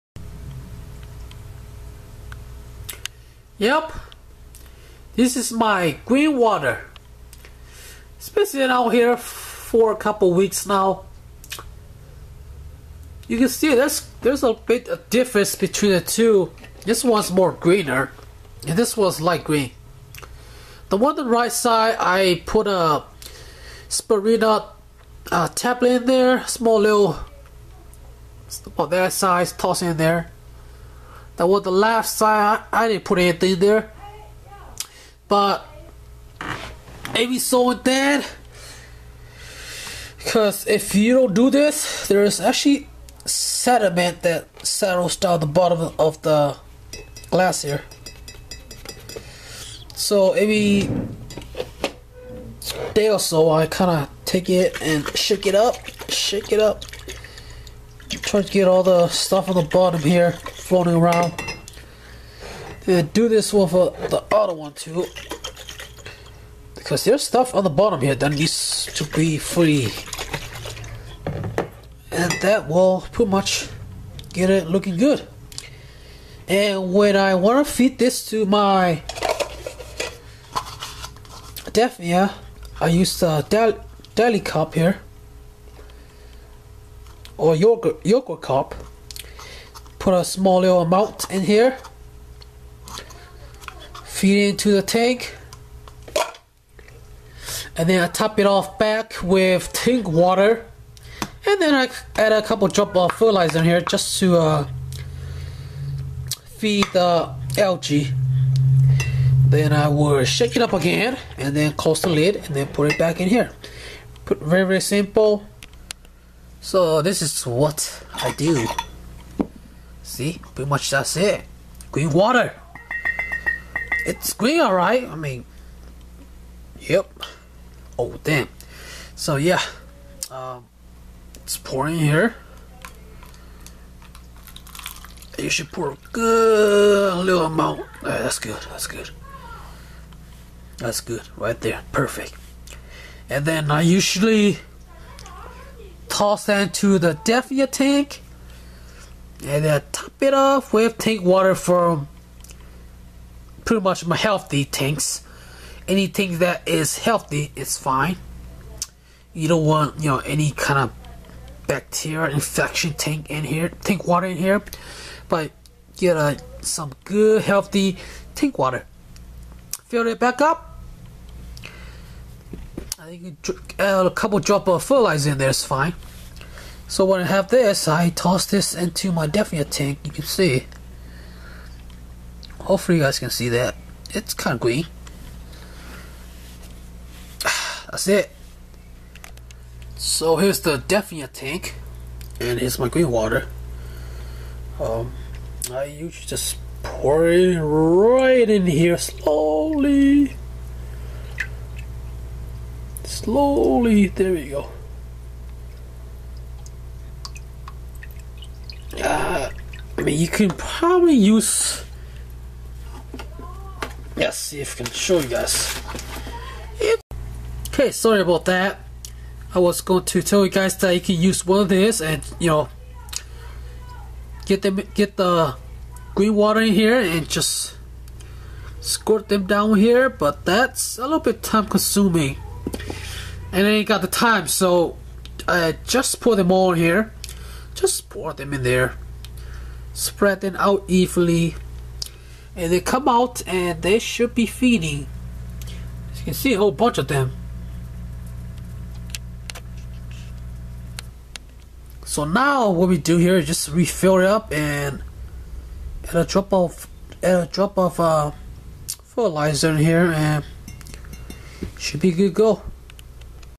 Daphnia Culture: Feeding Green Water
How I feed green water to my daphnia and refilling the bottle back up.